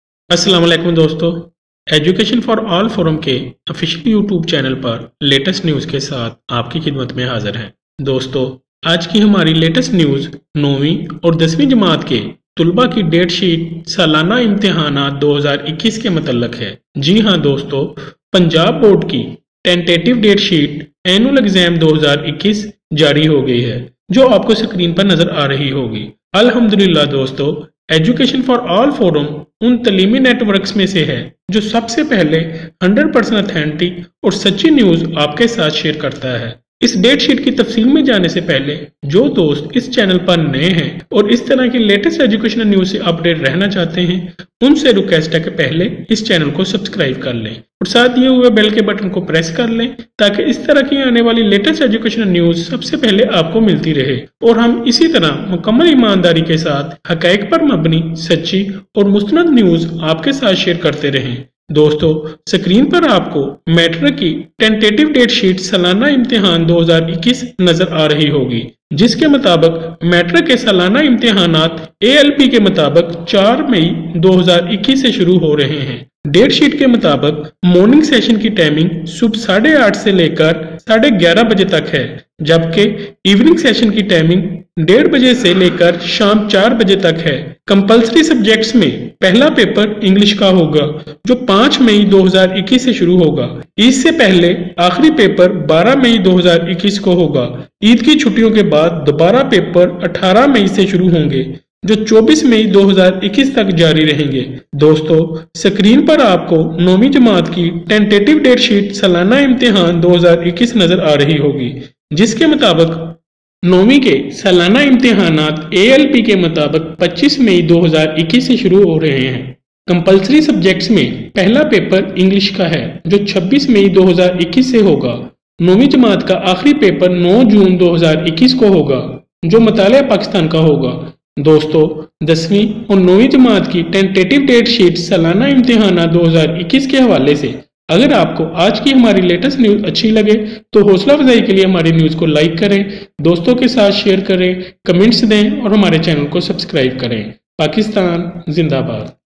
Good News! 9th & 10th Tentative Date Sheet Announced|Punjab Board Matric & Inter Date Sheet
Breaking News!Education Minister Latest Address To Students.Will Again Educational Institutes closed.